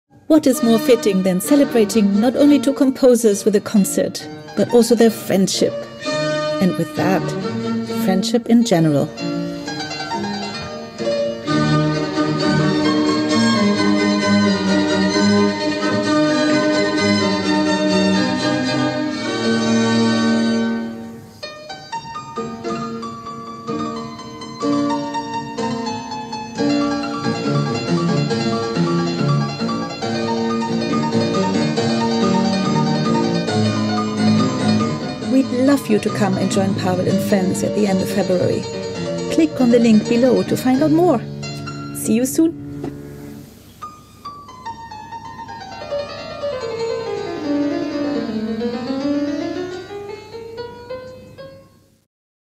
Haydn & Mozart fortepiano concertos with Pawel Siwczak and Bach Club Soloists
"Forgive me if I lose my head, but I love this man so dearly" wrote Joseph Haydn about his younger friend Mozart.

In this concert Bach Club Soloists are celebrating friendship and generosity of human spirit, with two fortepiano concertos played by a group of friends that inspire and challenge each other.

Pawel Siwczak @baroquepawel  will be playing a copy of Mozart's favourite Viennese instrument – come and hear it in London on 19 February 2022. We also aim to film this performance so that you can watch it later from your home anywhere in the world.